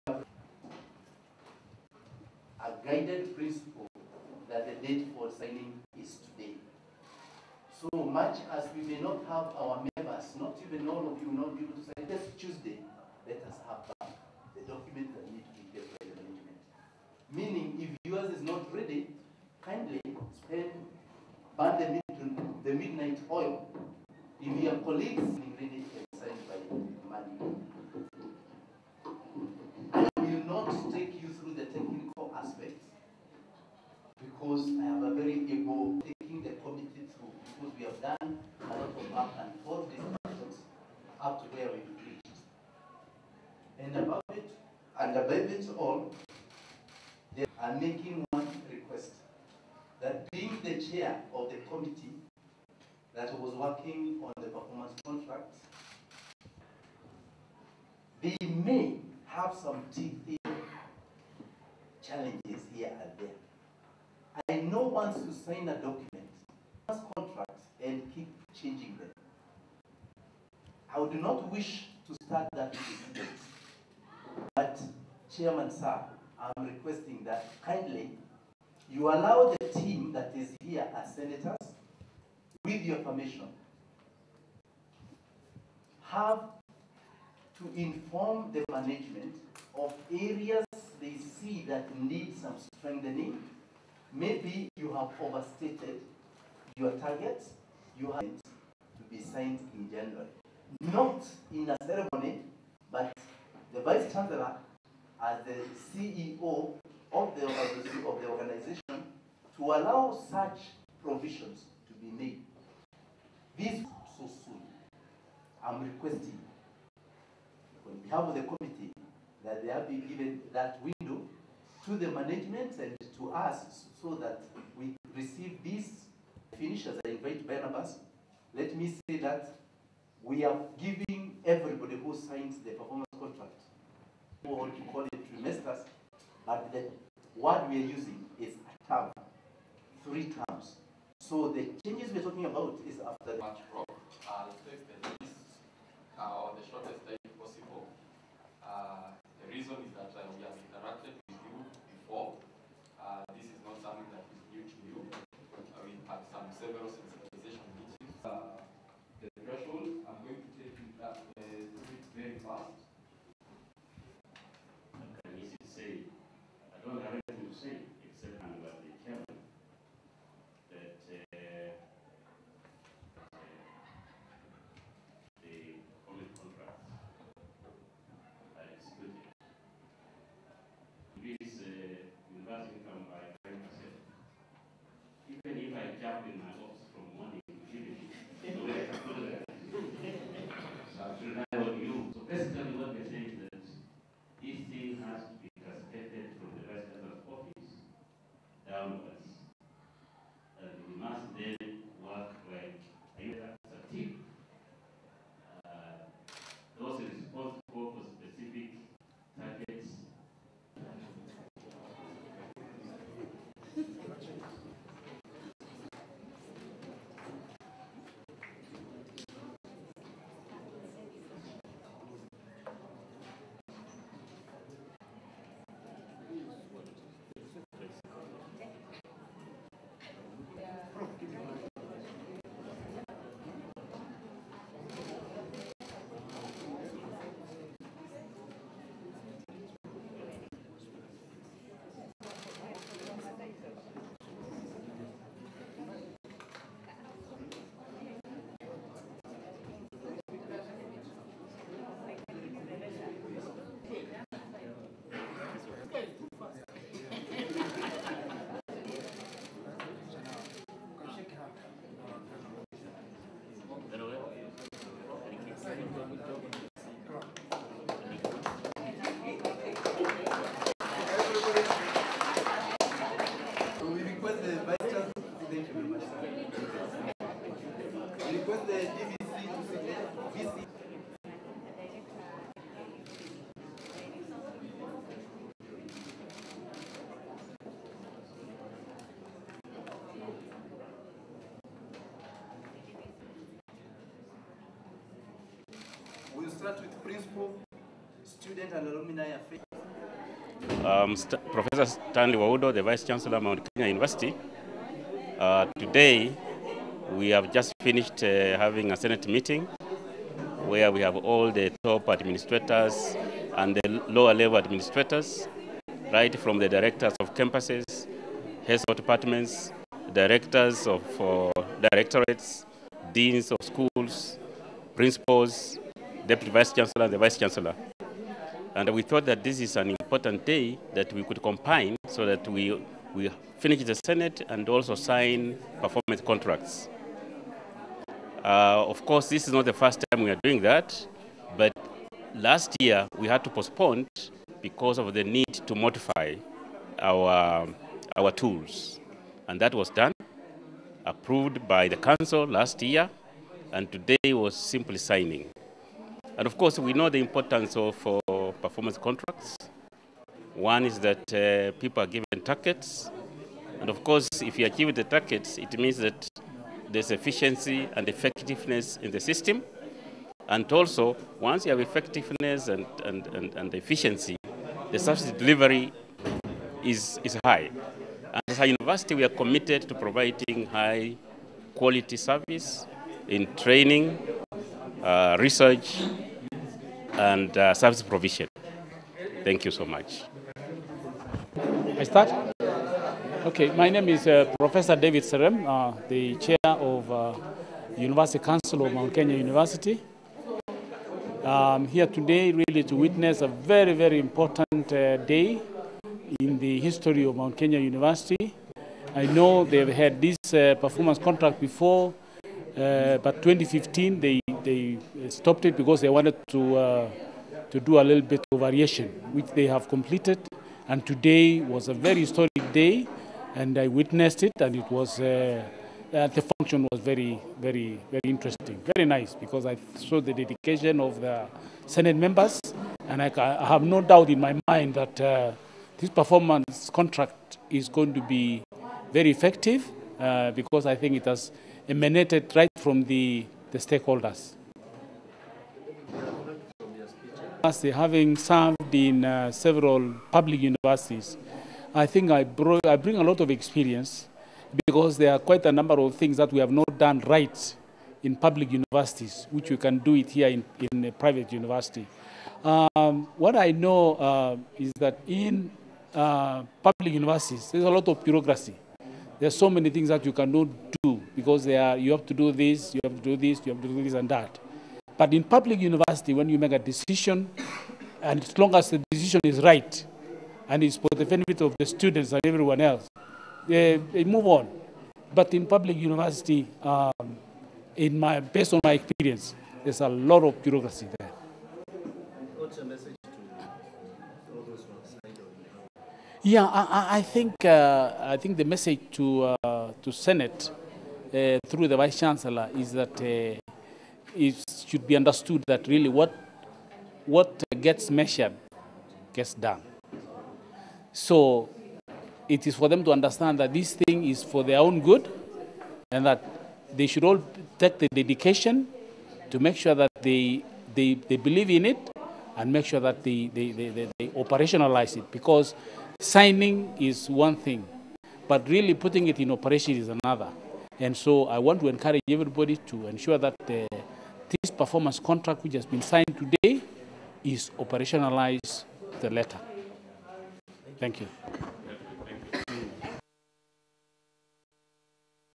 Mount Kenya University Staff Sign Performance Contract
Mount Kenya University top management signed their respective performance contract. Dr. David Serem, MKU university council chairman and senators witnessed the event.